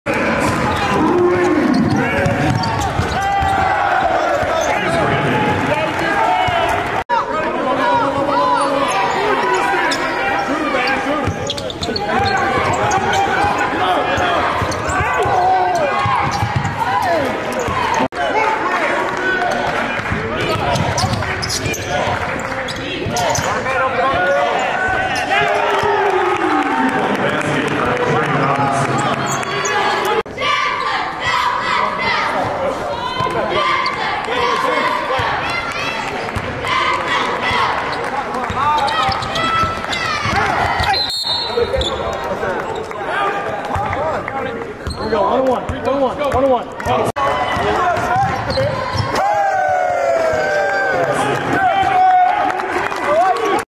Northern Kentucky Men's Basketball remains perfect in HLMBB play with 55-42 win over IUPUI
The Northern Kentucky men's basketball program tied a NKU NCAA DI era record with its 4-0 start to conference play, picking up a 55-42 win over IUPUI at the Indiana Farmers Coliseum on New Year's Eve.